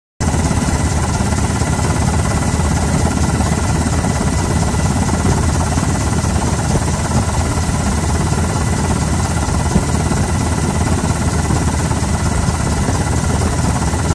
Aerial video of Hurricane Ida damage in Louisiana
The U.S. Coast Guard flew over parts of southeastern Louisiana, including Galliano, on Monday, Aug. 30 after Hurricane Ida made landfall Sunday as a Category 4 hurricane.